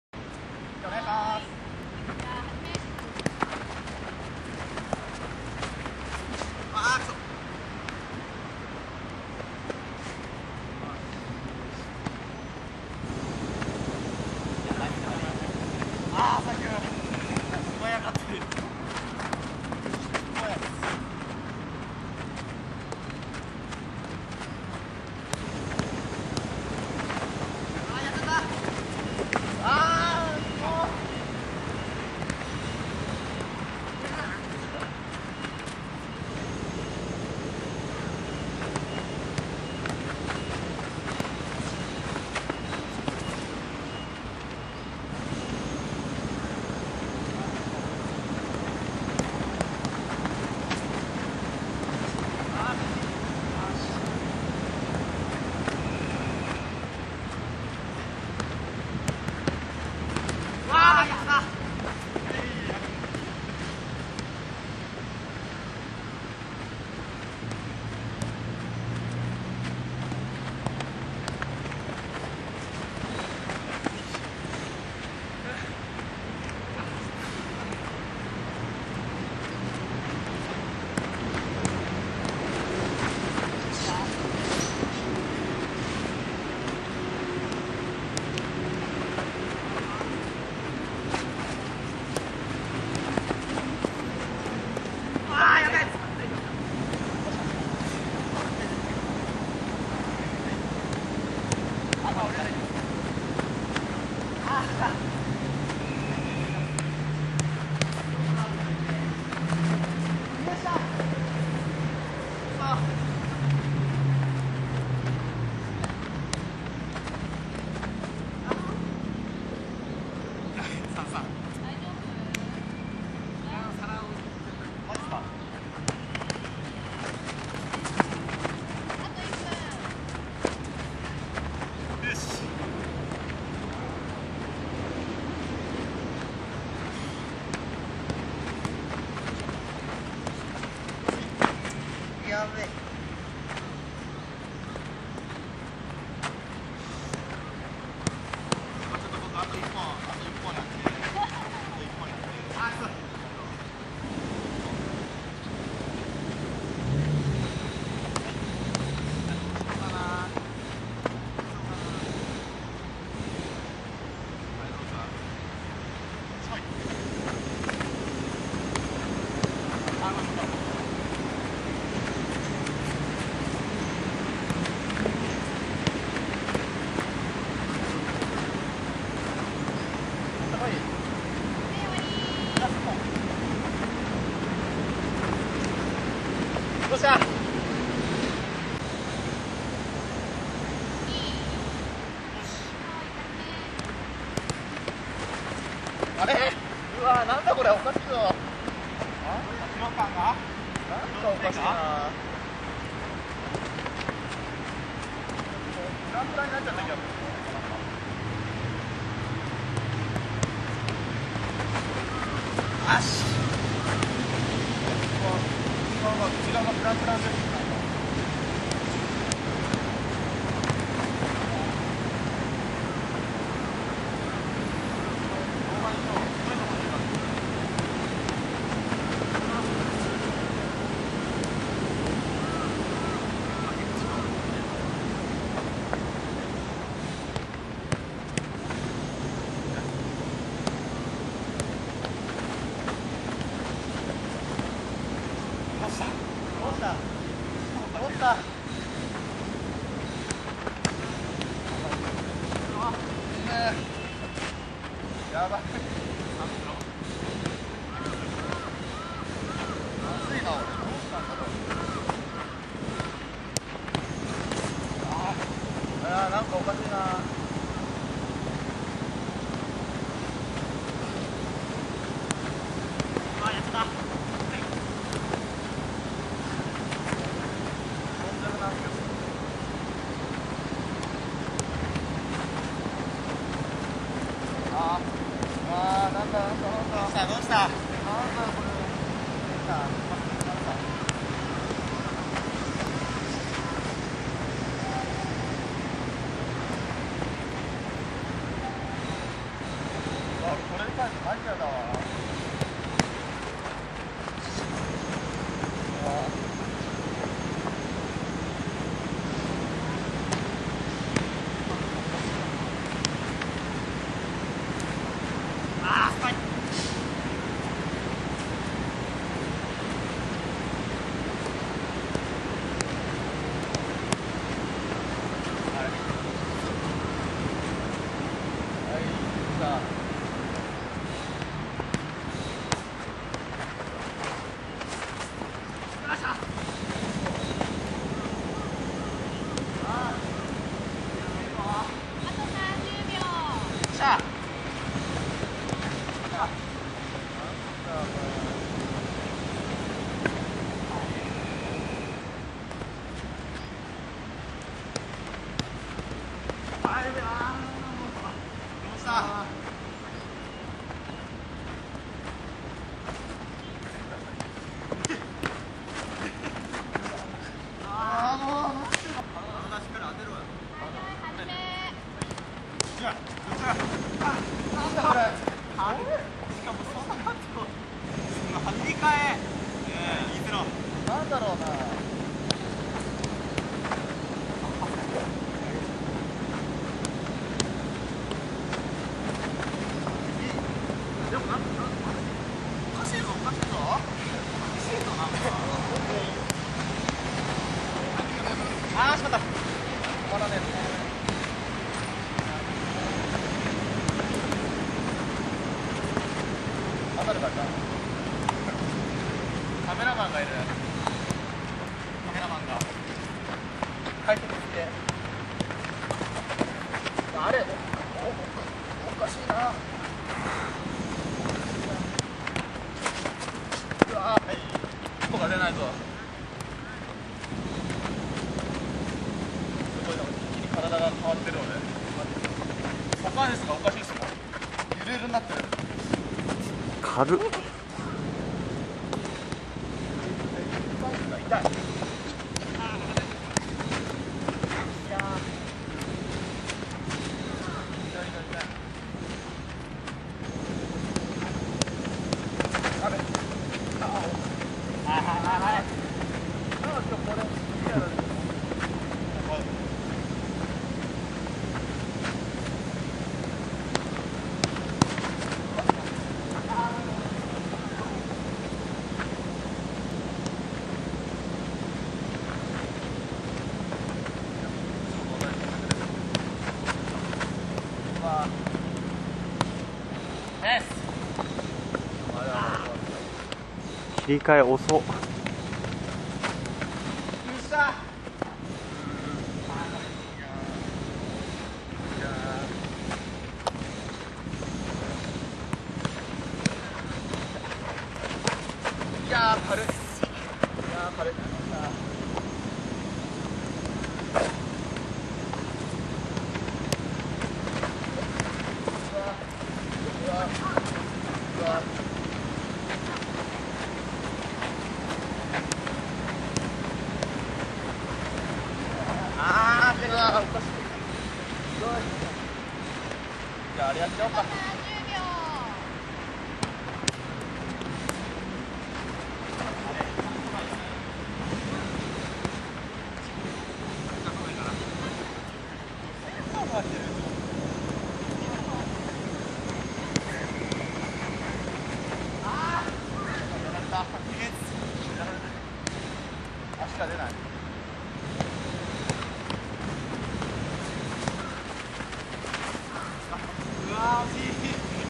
2010/6/22 AOKING式対人トレーニング：正方形の中で1vs1 その2
AOKINGの堀口祐輔が、少人数でも実施できる練習メニューを伝授致します。